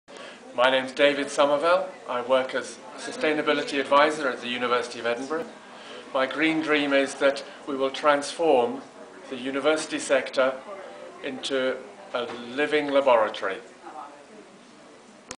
David's Green Dream
David's Green Dream is that the university sector can be transformed into a living laboratory.